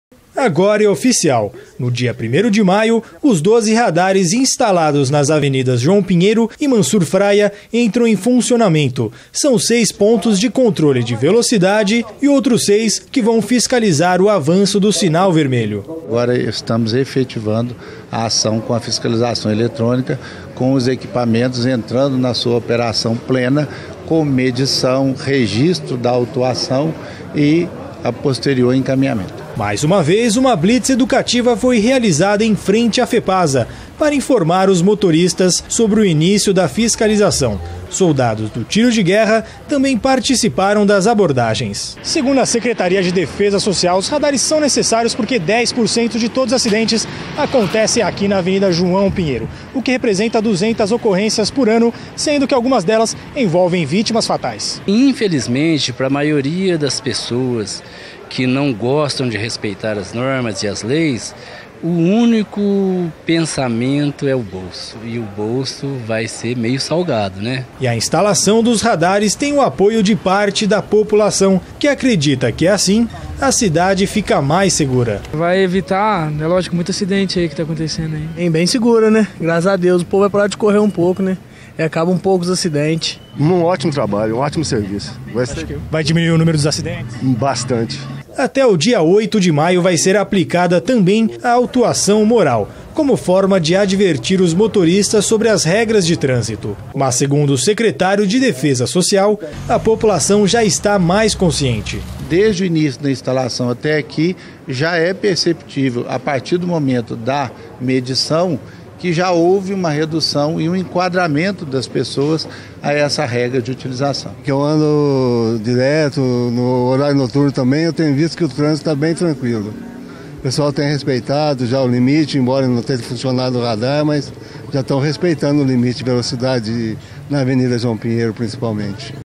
INÍCIO DAS ATIVIDADES DOS RADARES
Os motoristas que costumam ultrapassar o limite de velocidade na Avenida João Pinheiro devem ficar atentos. A partir de sexta-feira, dia primeiro de maio, terá início a fiscalização com radares.